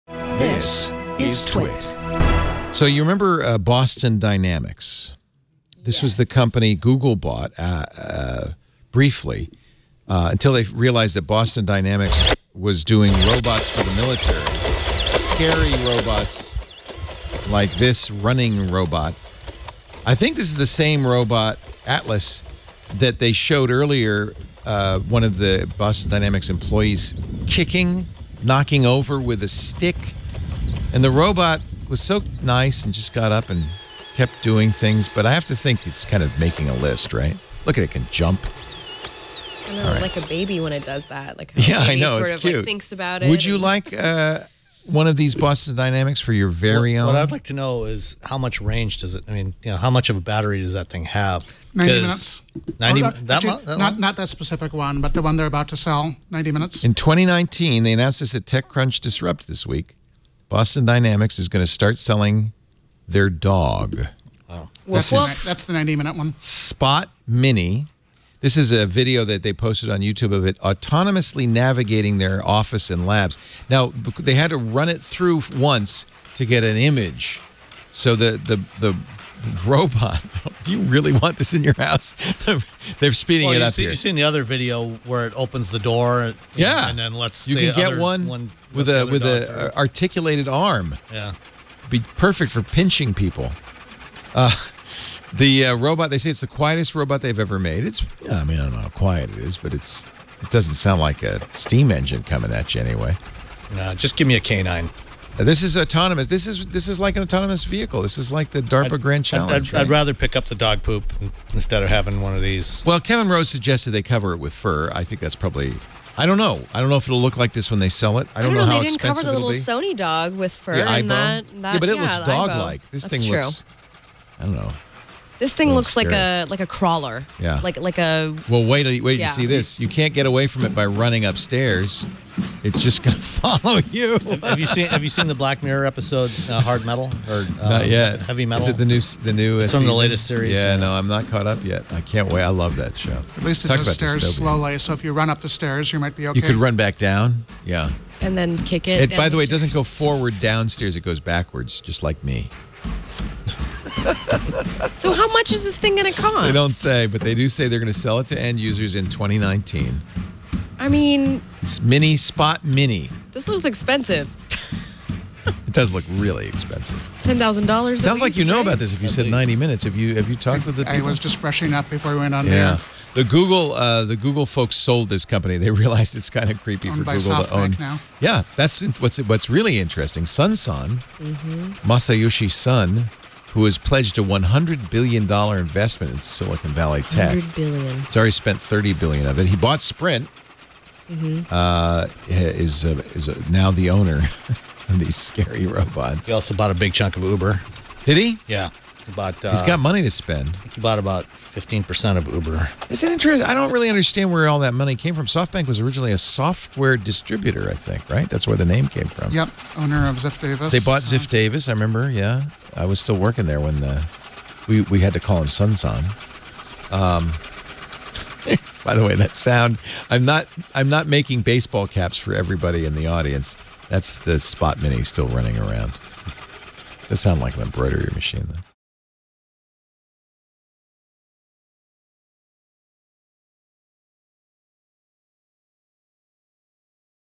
Boston Dynamics to Sell Spot Mini in 2019
Starting in 2019, you dreams of owning a pack of killer robot hunting dogs could finally come true.